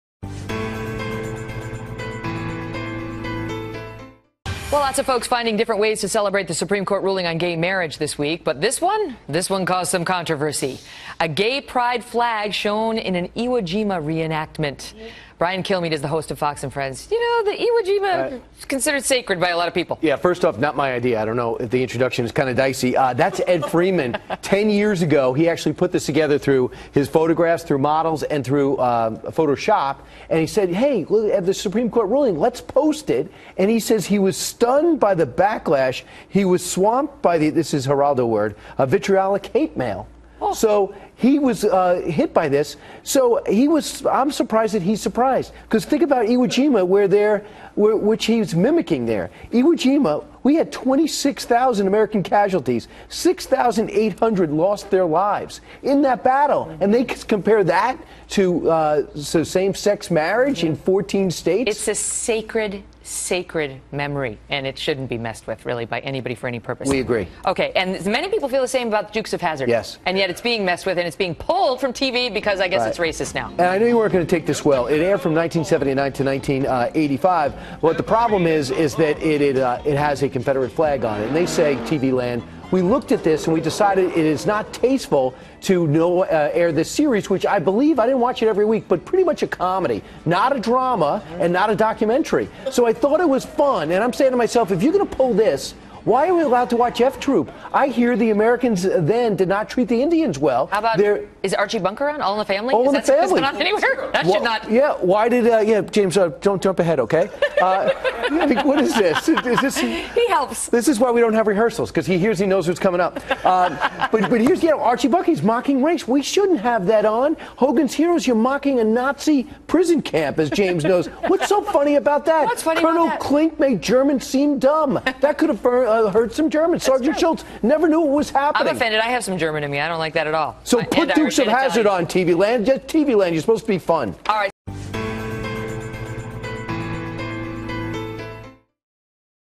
Iconic Iwo Jima Photo Recreated With Gay Pride Flag Sparks Outrage
The iconic photo of Marines raising the American flag over Iwo Jima in 1945 has been recreated with the gay pride flag.

The recreated image shows four muscular, skin-baring men raising a rainbow flag in a similar position that Marines did in the original photo.

Brian Kilmeade reported on "The Kelly File" tonight that the controversial photo was taken nearly 10 years ago and appeared on the cover of a gay magazine at the time.

The photo began recirculating last week after same-sex marriage was legalized nationwide.

Kilmeade said the photographer, Ed Freeman, was shocked by the hate mail and backlash he has received on social media.

"I'm surprised that he's surprised," Kilmeade stated.
Outrage over this photo "recreating" the iconic Iwo Jima photo of Marines raising the American flag during WWII -- comparing it to Friday’s Supreme Court decision in favor of gay marriage. What do you think about this??